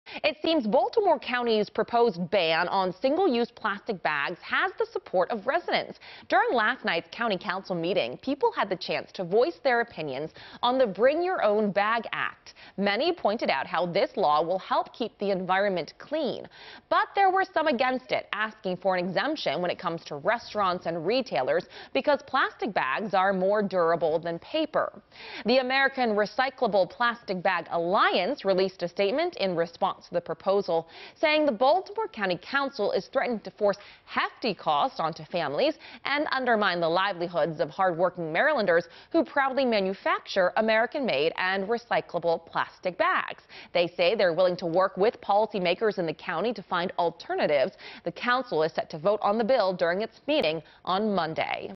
Residents voice opinions on proposed plastic bag ban in Baltimore County
Some residents expressed concerns on the ban, including cost and durability concerns.